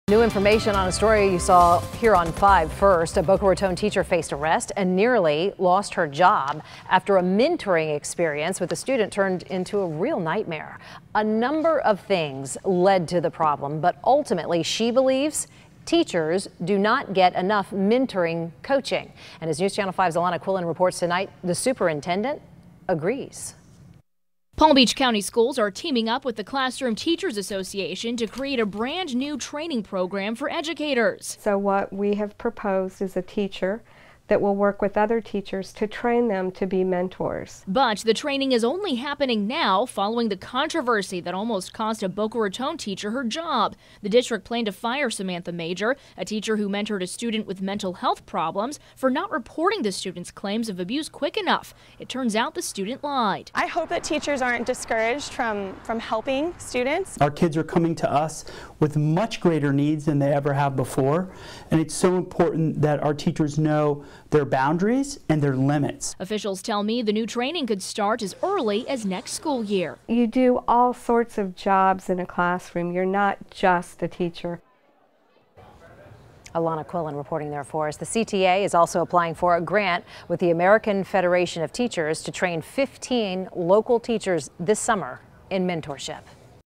Teacher to get student mentor training
A Boca Raton teacher faced arrest and nealry lost her job after mentoring experience with a student